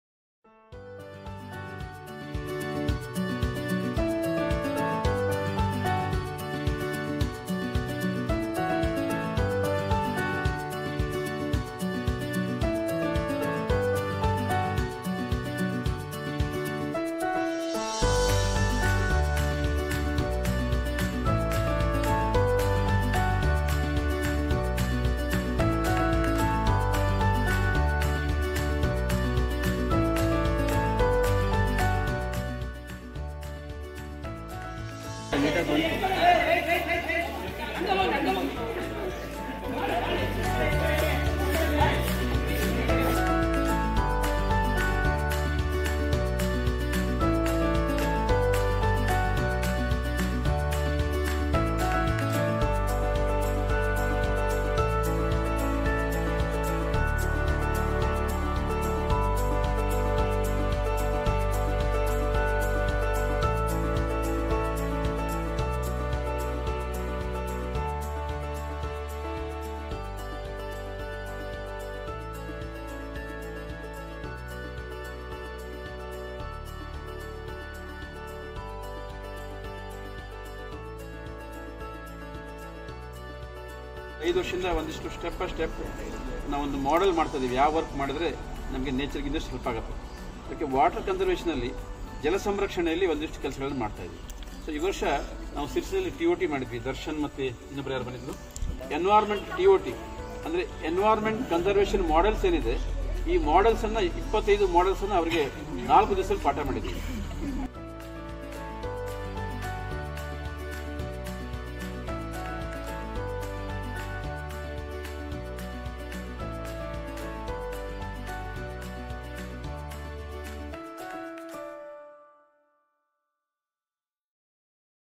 Kalyani Cleaning Activity - Tumakuru
Youth for Seva, Tumakuru team organised Kalyani Cleaning activity in Jayamangalli River Birth place, Devarayanadurga.  In this activity, YFS environment state coordinator Shree Umapathi Bat Sir, Environmentalists Hariprasad Nadiger, Siddapa Ji, And  Vidyodaya Law college NSS officer Kishor Sir and G H Ravikumar Sir,  Electrical department Shreedevi Institute of Technology participated in this activity.  Around 85 volunteers participated in this activity.